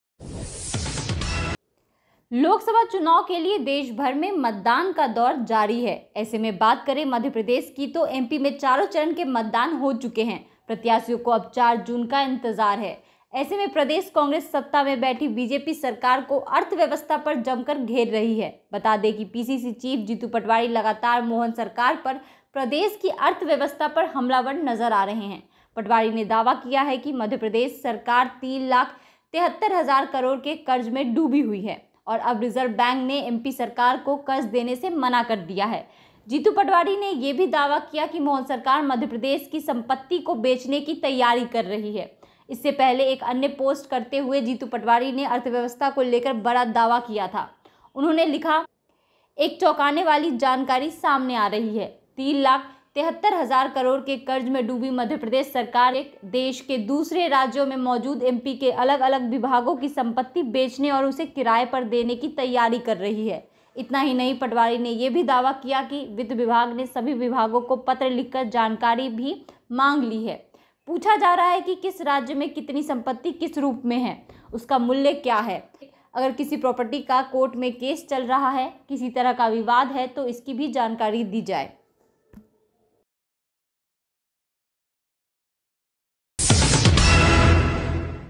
जीतू पटवारी के दावे से गरमाई सियासत
लोकसभा चुनाव के लिए देश भर में मतदान का दौर जारी है ऐसे में बात करें मध्य प्रदेश की तो एमपी में चारो चरण के मतदान हो चूके हैं प्रत्याशियो को अब 4 जून का इंतजार है एसे में प्रदेश कांग्रेस सत्ता में बैठी बीजेपी सरकरा को अर्थव्यवस्था पर जमकर घेर रही है बता दें की पीसीसी चीफ जीतू पटवारी लगातार मोहन सरकार पर प्रदेश की अर्थव्यस्था पर हमला बर नजर आ रहै हैं पटवारी ने दावा किया है की मध्य प्रदेश सरकार 3 लाख 73 हजार करोड़ के कर्ज में डूबी हुई है, और अब रिजर्व बैंक ने एमपी सरकार को कर्ज देने से मना कर दिया है